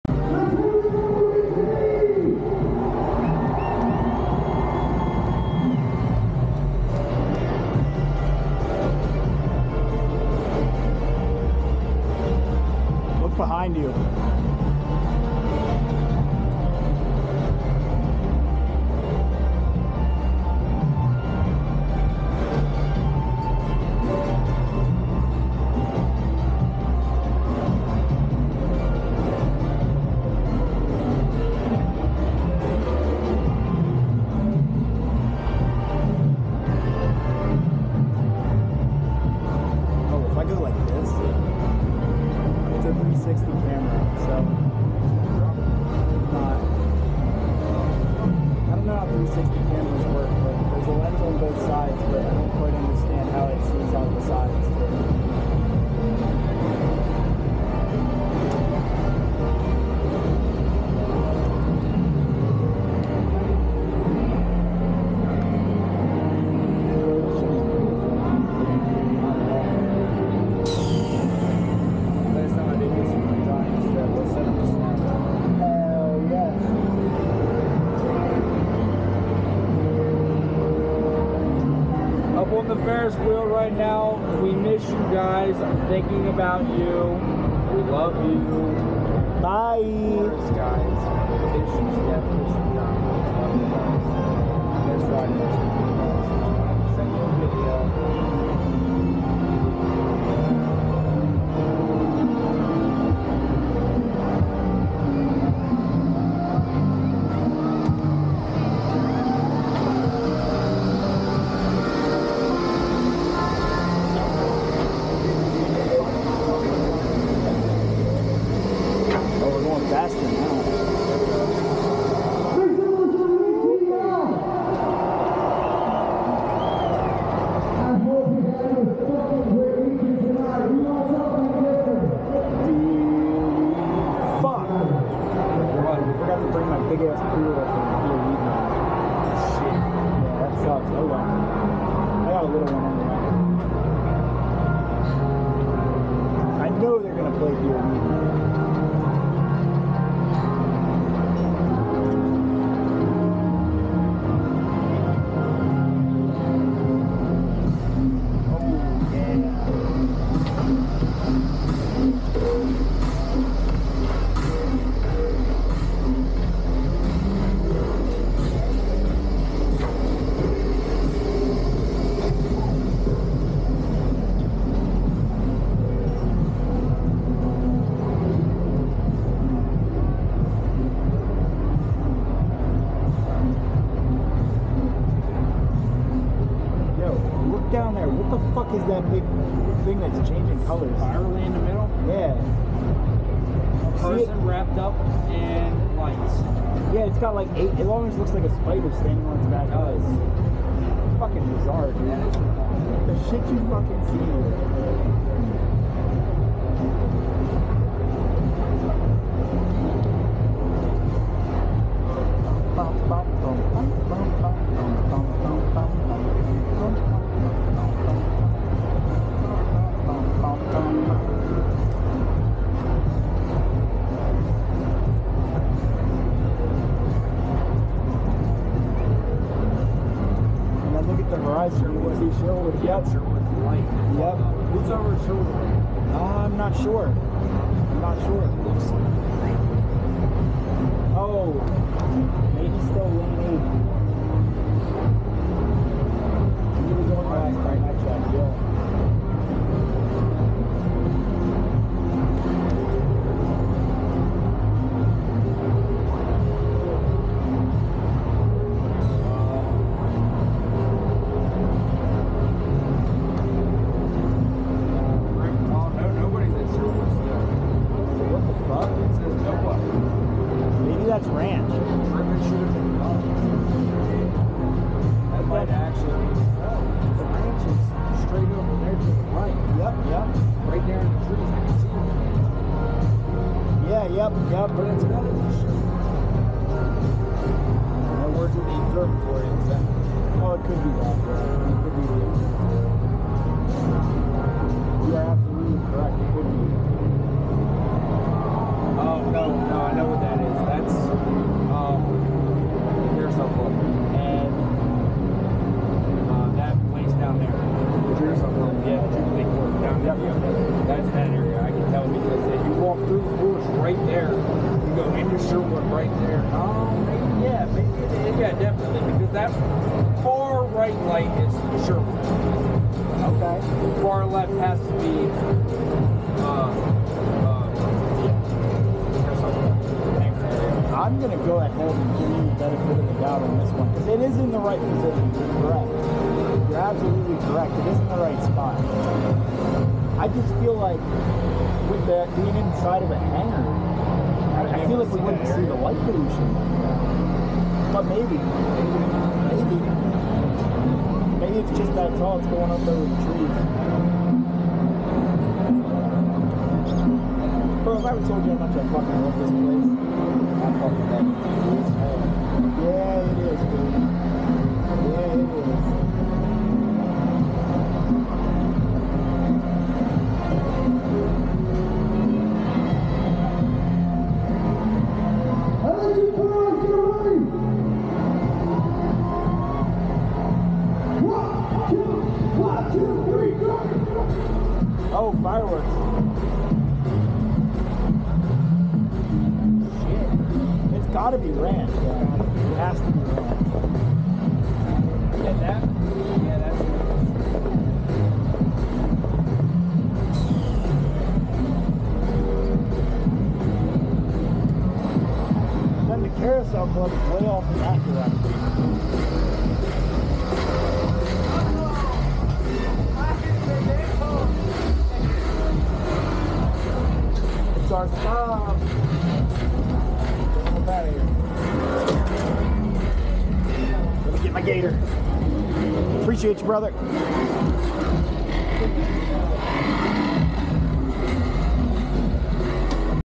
Electric Forest `23 (FERRIS WHEEL RIDE) in 360
The Electric Forest always has a Ferris wheel right at the entrance, and I asked my friend to take my 360 camera with him when he took a ride.
- Huge shout out to Brett for taking the camera with him, and taking such good care of it!

(This was recorded on Saturday 6/24/23)